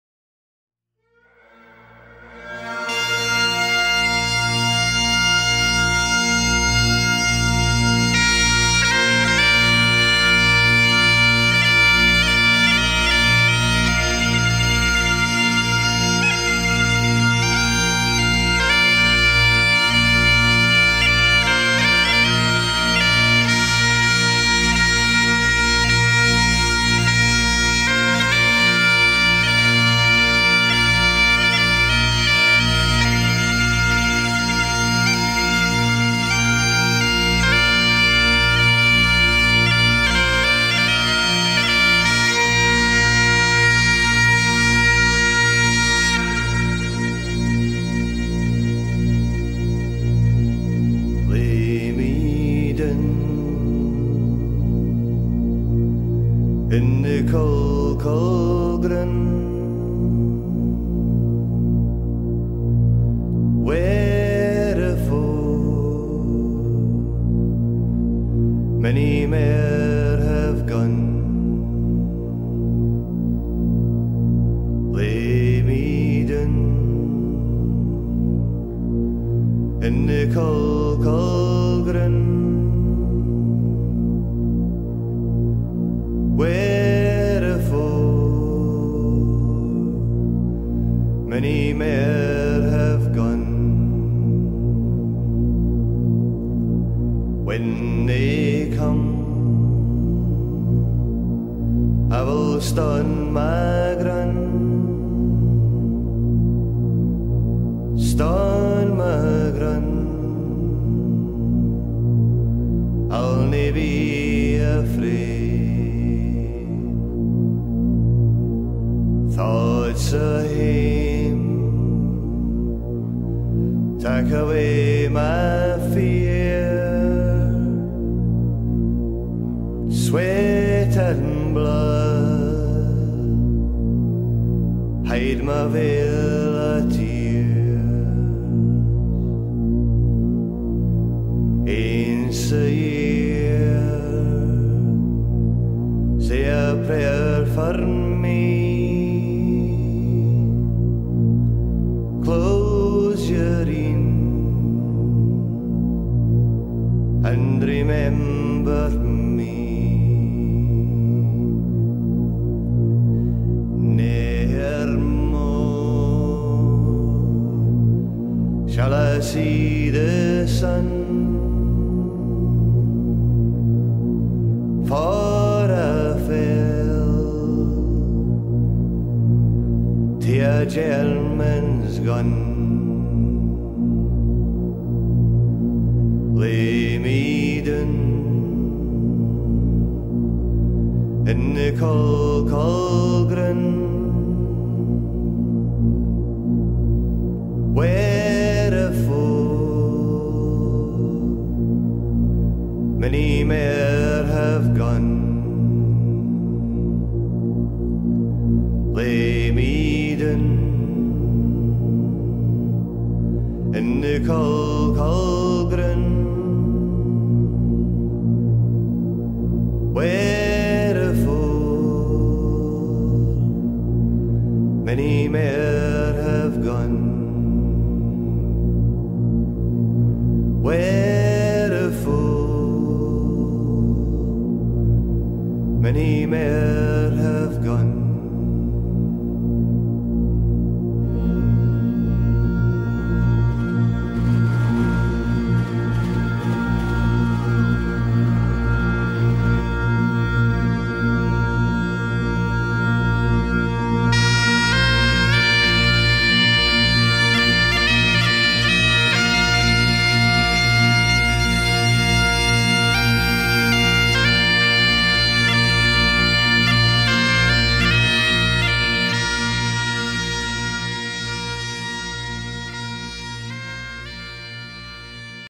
RAF in Afghanistan 2006 - 2007
A small movie of some pictures of the RAF and NATO forces in Afghanistan my rememberence tribute.